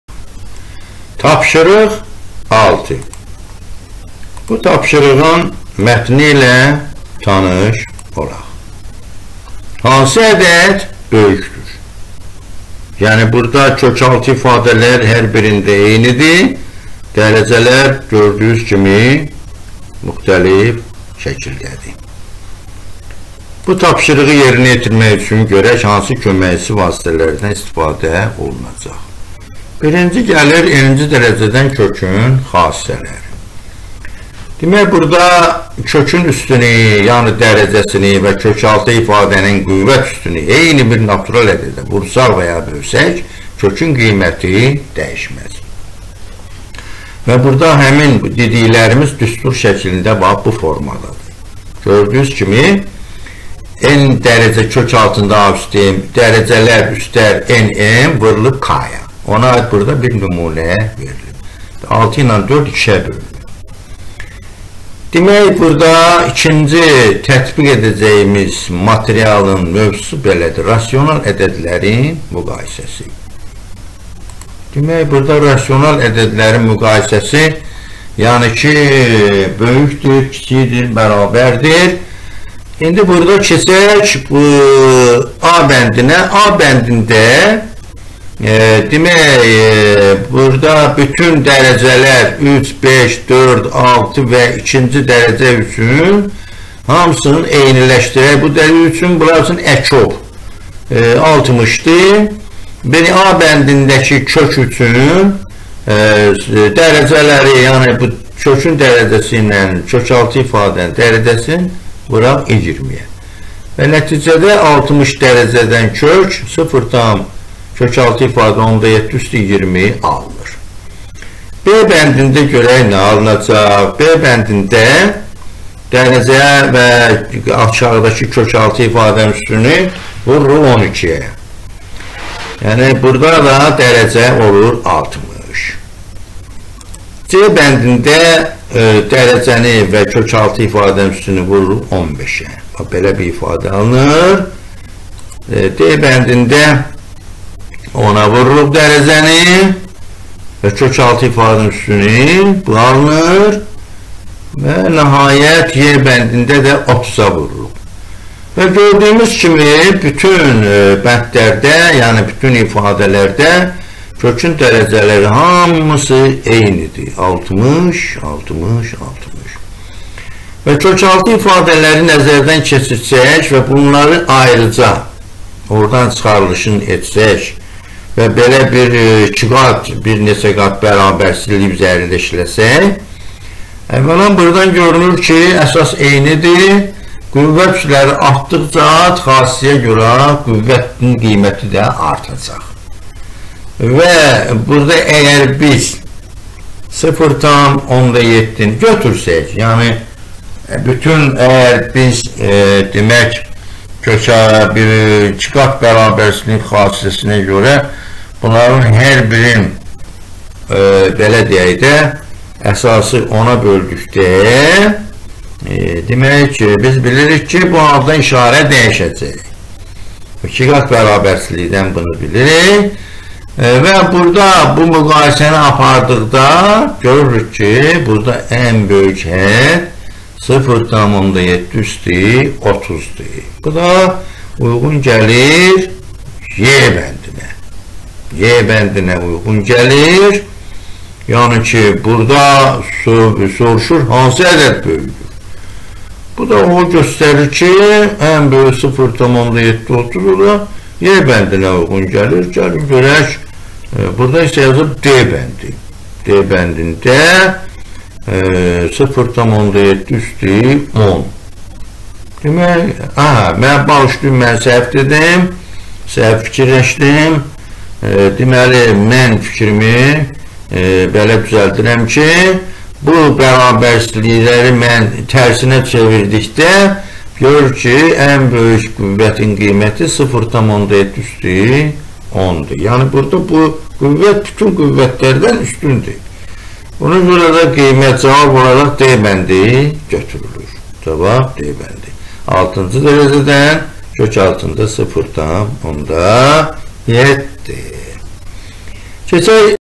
Vid izah S 2 6IX sinif.Riyaziyyat.Buraxılış.Mövzu sınaq-1.6.n-ci dərəcədən kök.Ras üstlü qüvvət.
IX sinif buraxılış imtahanına hazırlıq.
bütün siniflər üzrə mövzu sınaqlar burada. daxil ol imtahan ver.Biliyini yoxla.